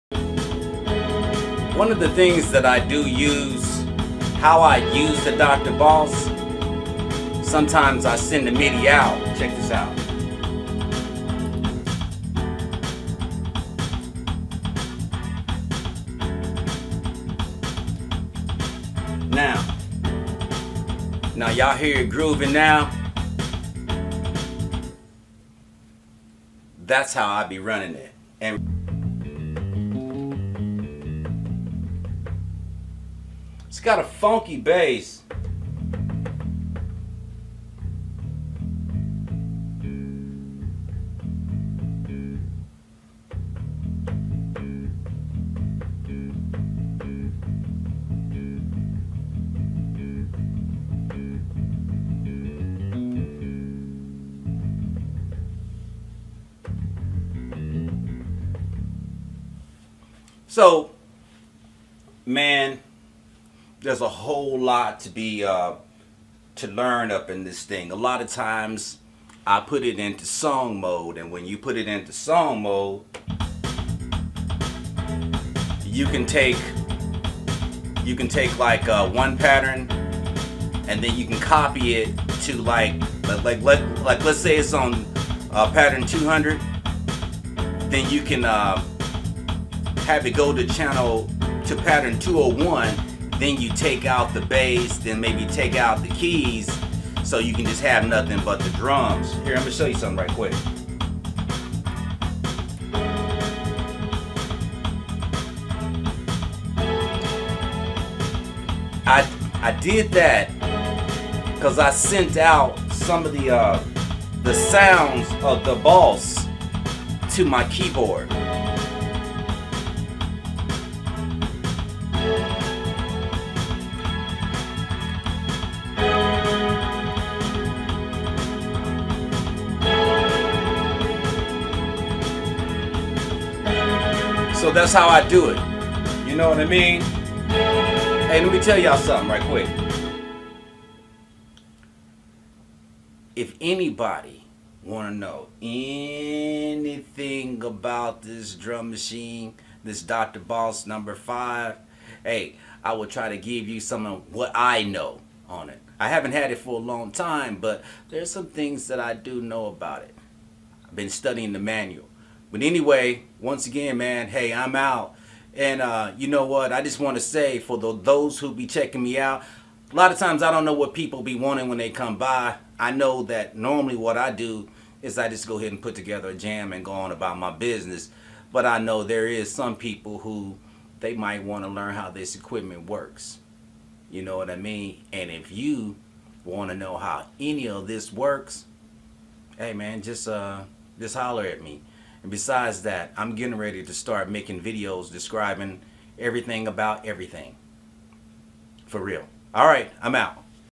How I construct Music with My BOSS DR 5
In this video, I am talking about the BOSS DR-5.  So I am talking about how I use the parts of the preset pattern and remix the sounds. And also I run different midi channels to different keyboards. The Drum machine might not look Like much, but it is made with the musician in mind.